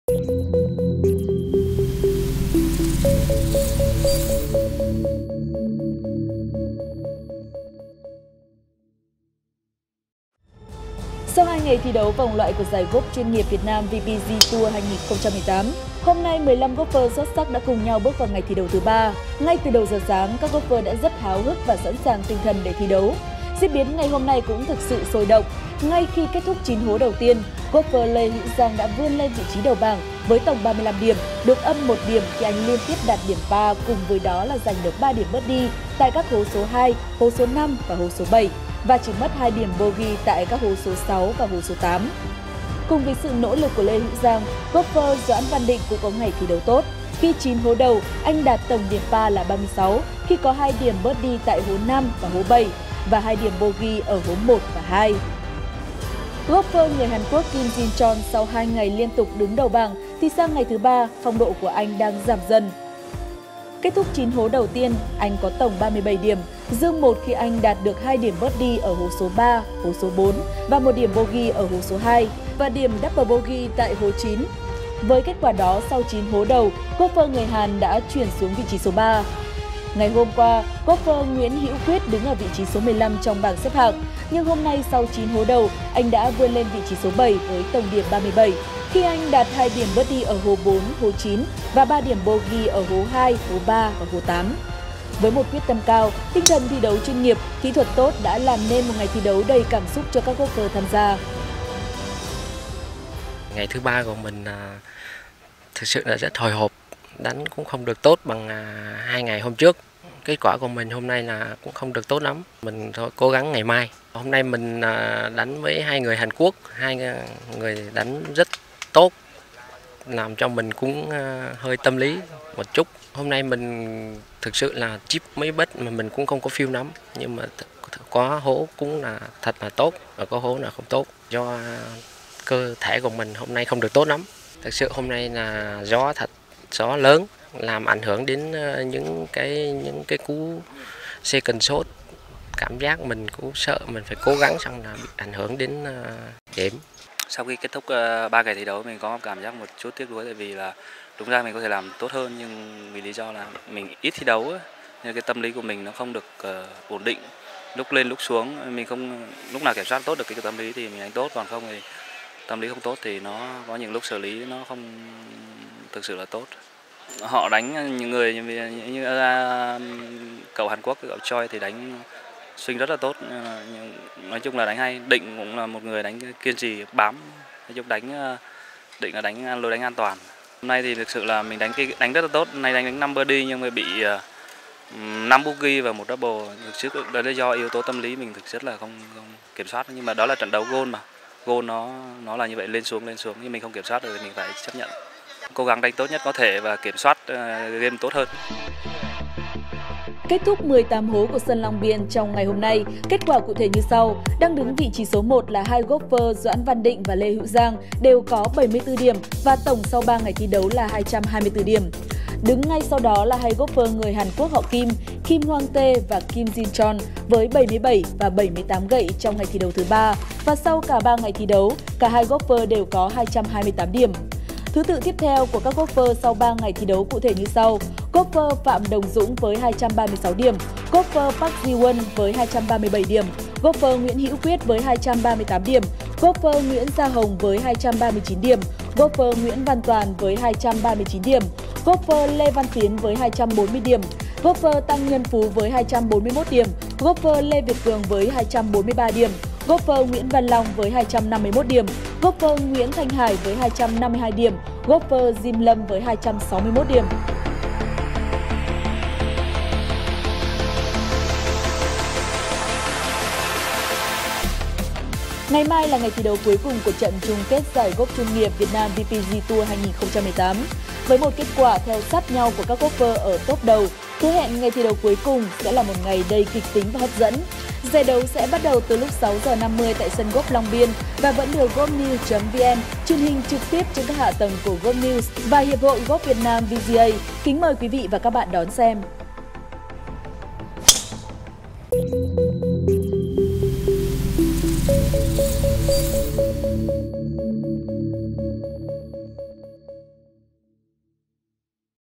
Ngày thi đấu thứ 3 vòng Chung kết của Giải golf chuyên nghiệp Việt Nam VPG Tour 2018
Sau 2 ngày thi đấu của vòng loại giải golf chuyên nghiệp Việt Nam VGA Tour 2018 hôm nay 15 golfer xuất sắc đã cùng nhau bước vào ngày thi đấu thứ 3. Ngay từ đầu giờ sáng các golfer đã rất háo hức và sẵn sàng tinh thần để thi đấu.